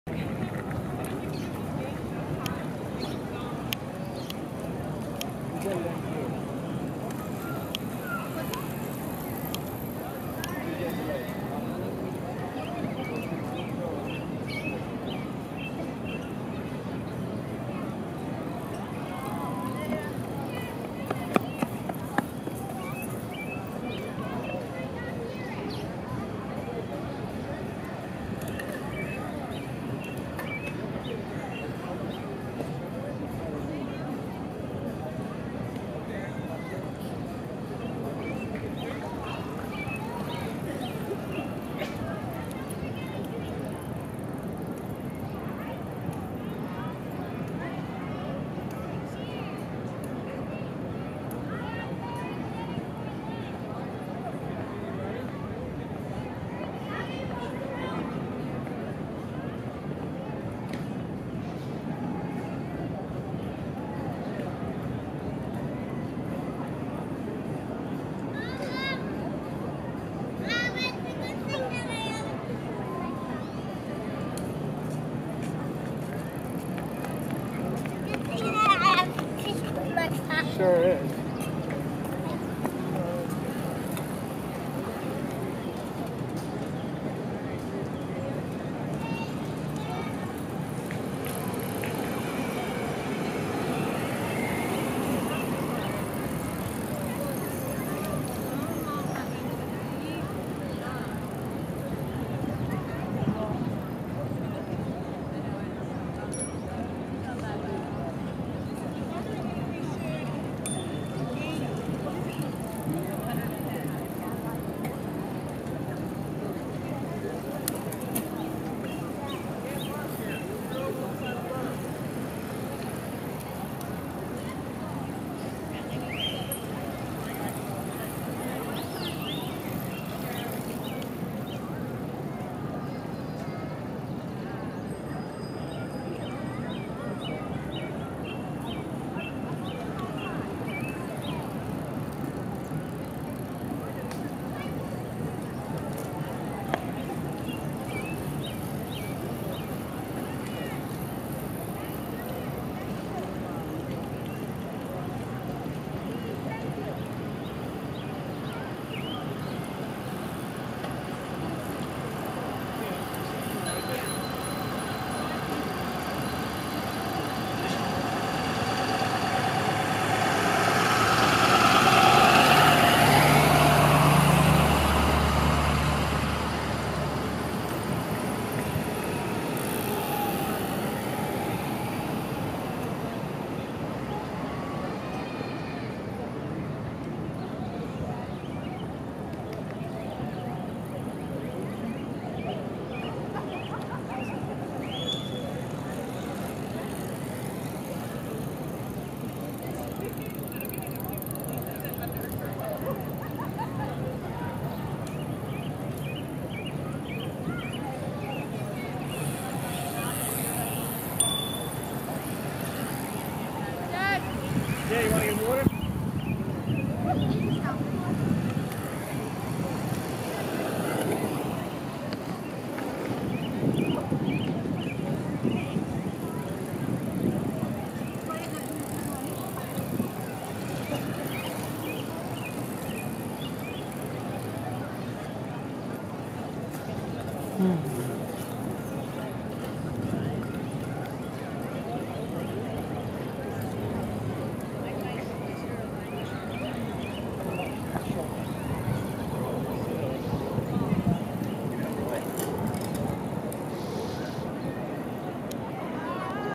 Central Park - GH4 4K Test
Here is some 4K footage straight out of the GH4. I recorded it in Cinema 4K 24fps mode. I didn't modify it at all besides uploading it to YouTube. Sorry it's a little shaky and sometimes defocuses. That's because I was testing out the continuous autofocus and the Power OIS in the lens. The lens, by the way, is the Panasonic 12-35. The footage was taken on the park drive in Central Park NYC right near Cleopatra's Needle, which can be seen in the background.